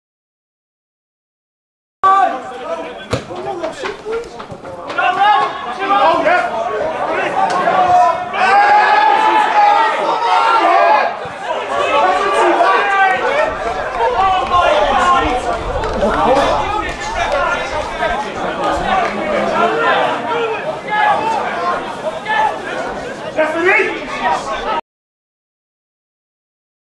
YOU CAN WHINE ALL YOU WANT! (Atherton LR Vs South Liverpool)(08.08.23)
South Liverpool FC were incensed that they were not awarded a penalty midway through the first half against Atherton LR, and they had absolutely no right to be!  Benni Wilkinson executing a fine tackle to thwart the visitors attack.